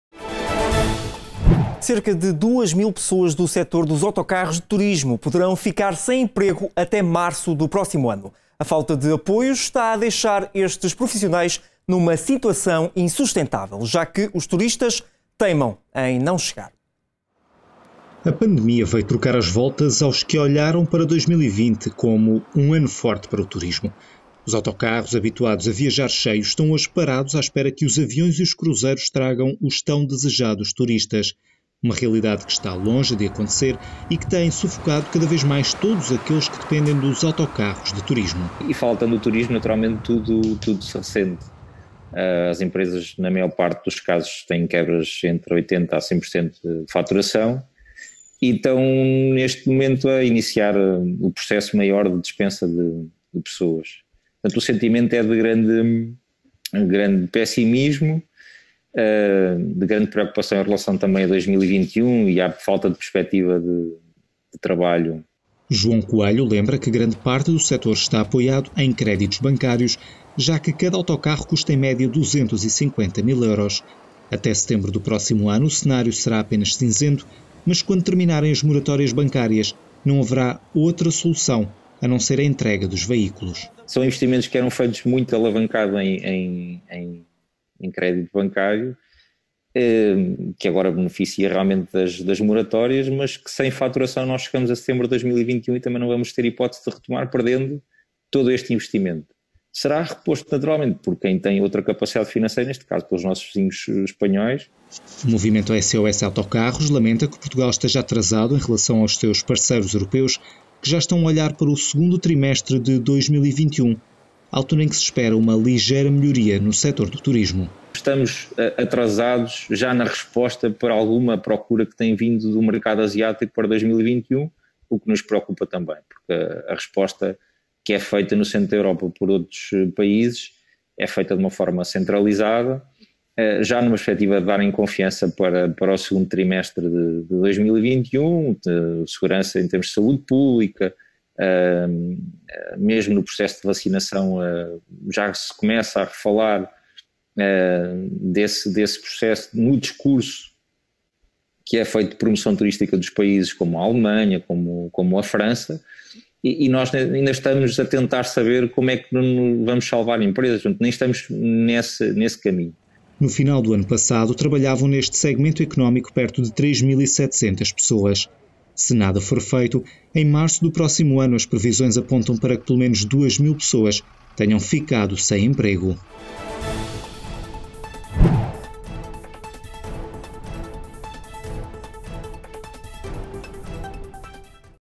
Crise nos autocarros de turismo
Cerca de duas mil pessoas do setor dos autocarros de turismo poderão ficar sem emprego até março do próximo ano. A falta de apoios está a deixar estes profissionais numa situação insustentável... já que os turistas teimam em não chegar.